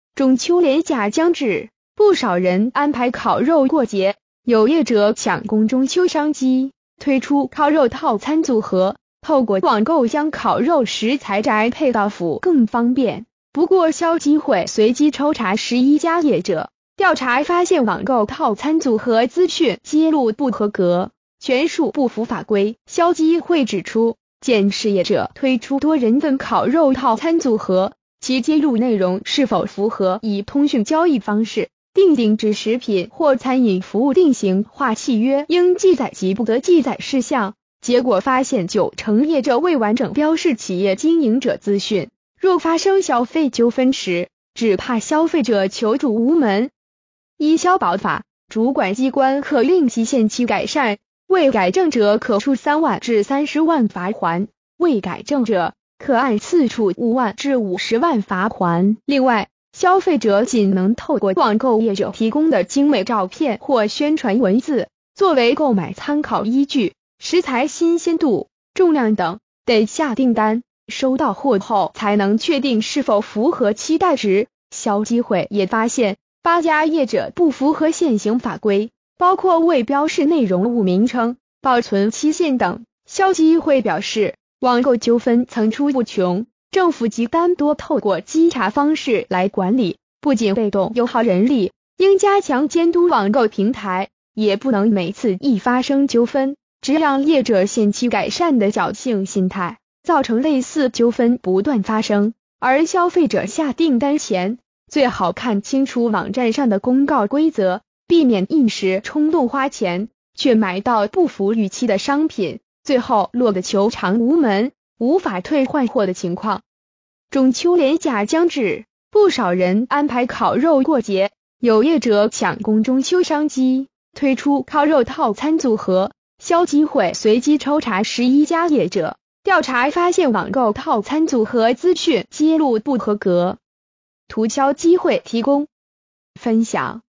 網購中秋烤肉餐 消基會抽查不合格恐求償無門
中秋連假將至，不少人安排烤肉過節，有業者搶攻中秋商機，推出「烤肉套餐組合」，透過網購將烤肉食材宅配到府更方便，不過消基會隨機抽查11家業者，調查發現網購套餐組合資訊揭露不合格，全數不符法規。

發表評論評論如下。